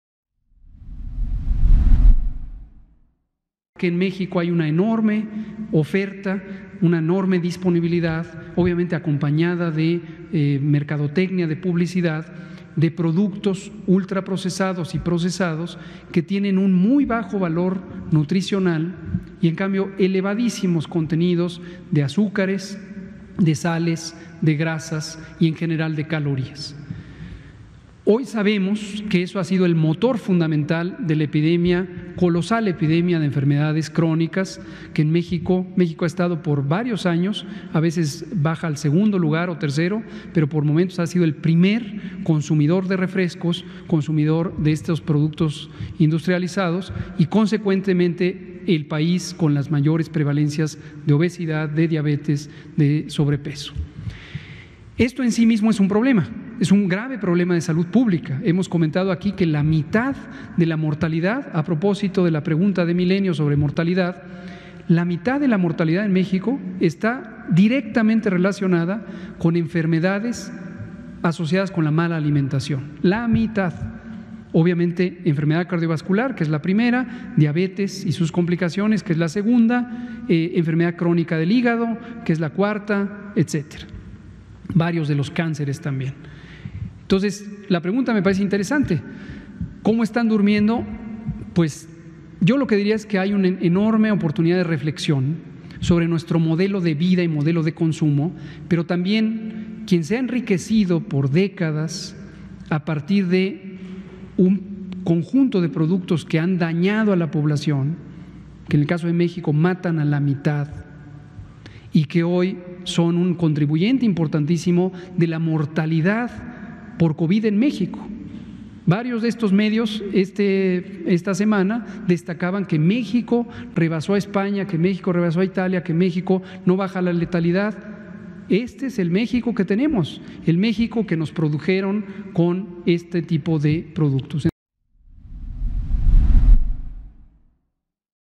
No es culpa de López-Gatell la alta mortalidad por Covid-19.
👉La opinocracia culpa a López-Gatell de las muertes de Covid-19.
¿Dónde estuvieron, todos estos años que nuestro país se convirtió en el primer consumidor de refrescos, primeros lugares diabetes y obesidad?